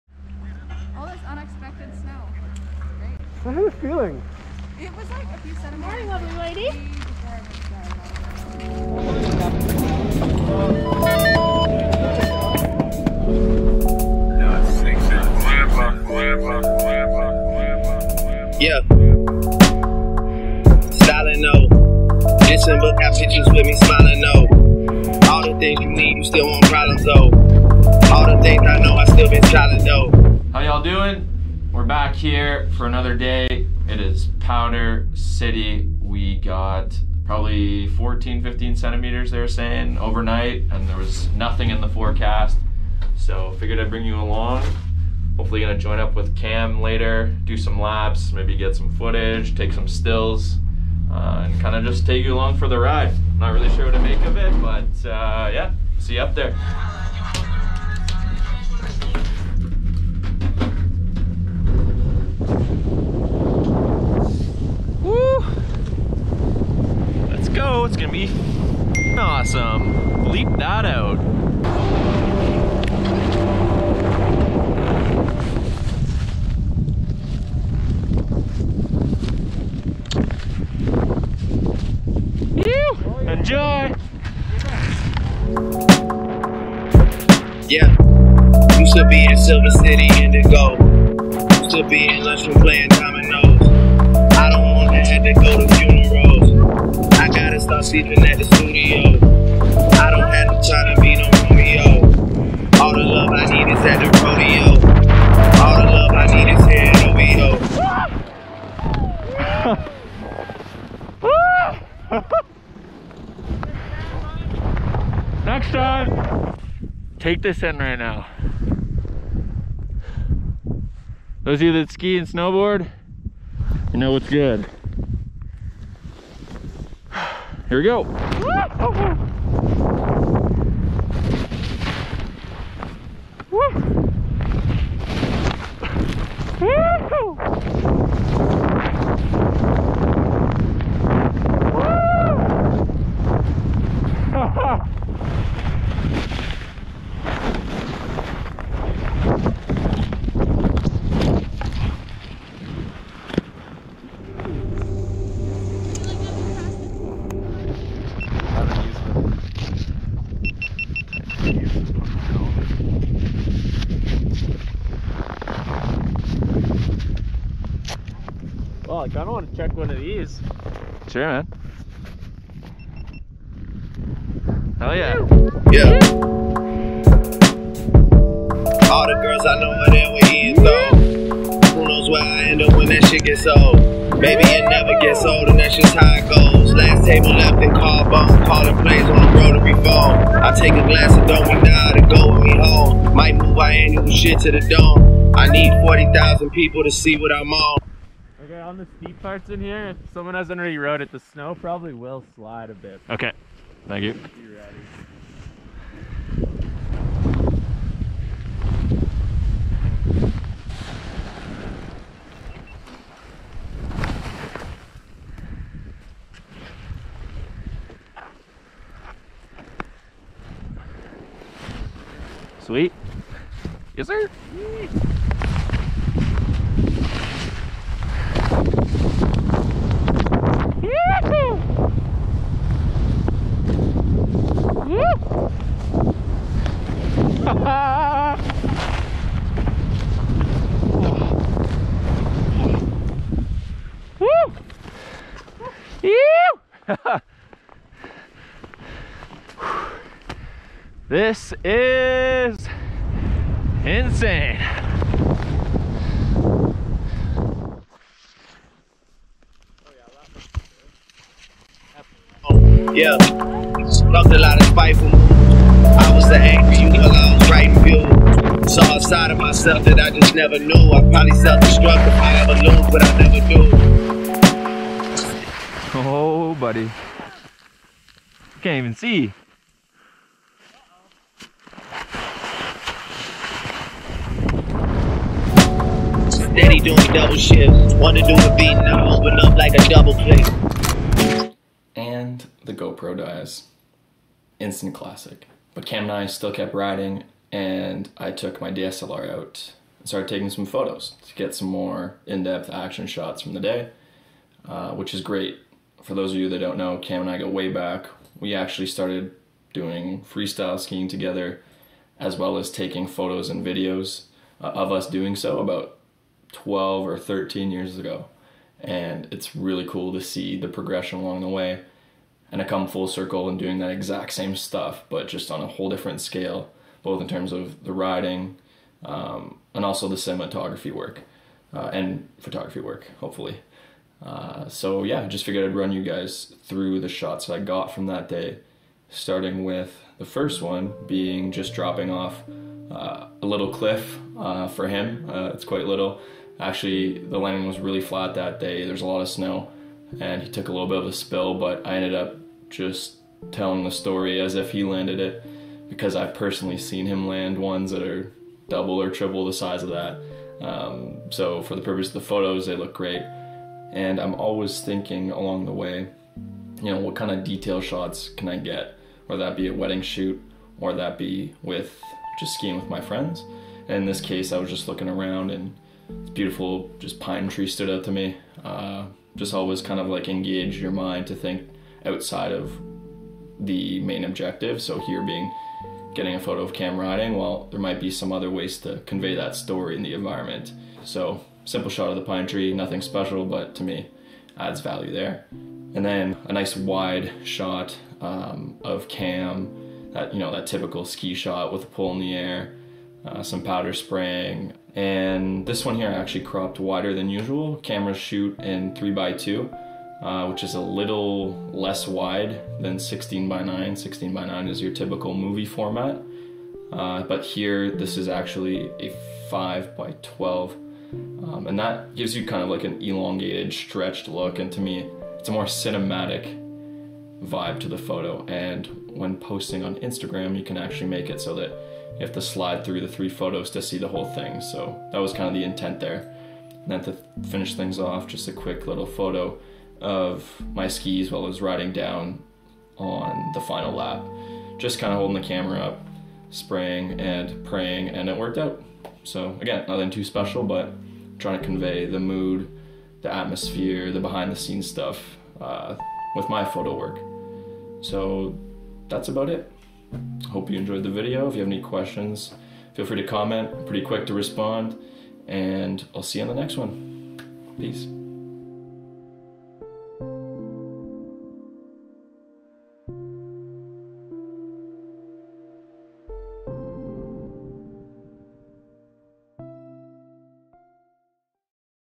Skiing FRESH POW at Revelstoke Mountain Resort
I always get excited when I get to ski fresh powder and take photos in the same day! In this video, I link up with my good childhood friend Cam Keith (@camk3ith) for some laps around Revelstoke Mountain Resort and then take you through some of the images I captured from that day.

I'm new to being in front of the camera, so any feedback would be greatly appreciated!

- Maxim B. Vidricaire

💍 Hire me for your wedding across Canada!